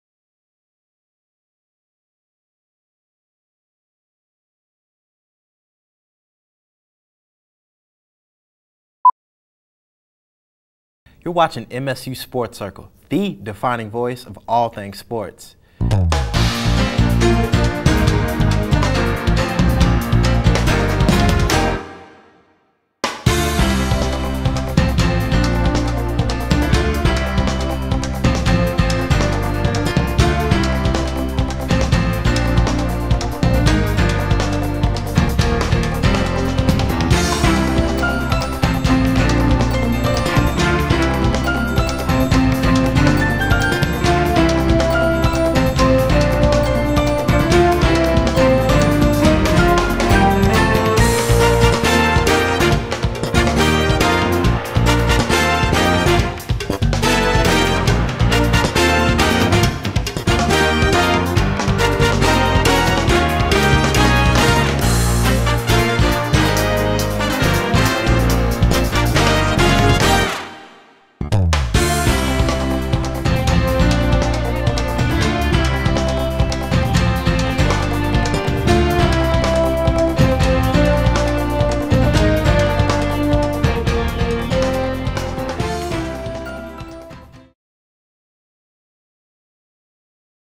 MSU Sport Circle intro
Morehead State University's 1st sports show made and ran by students such as myself and others. Enjoy!